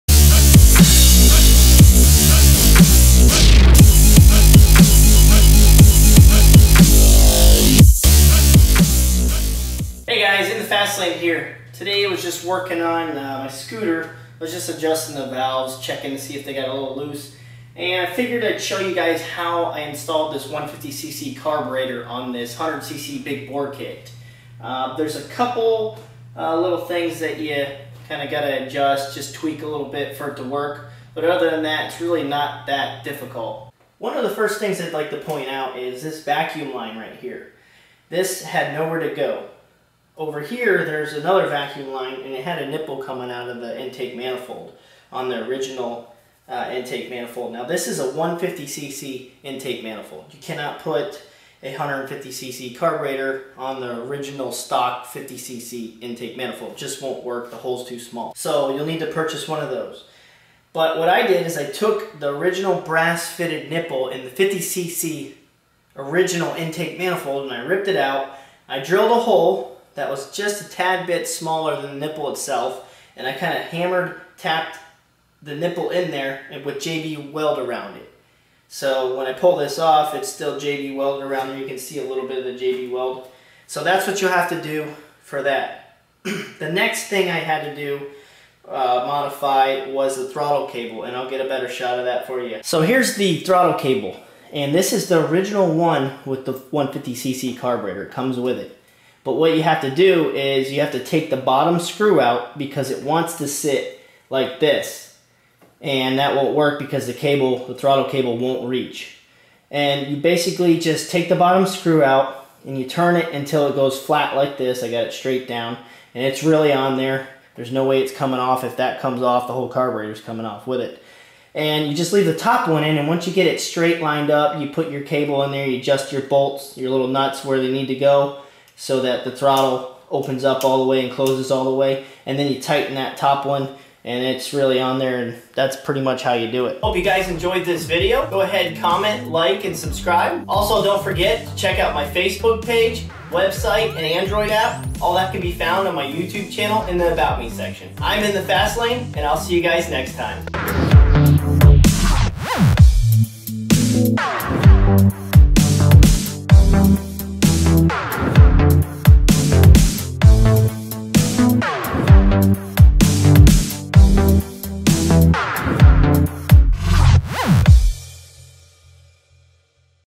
150cc Carburetor On 50cc Gy6 "How To"
In this video I show in detail how to fit 150 Gy6 Carburetor on a 50cc-100cc BBK.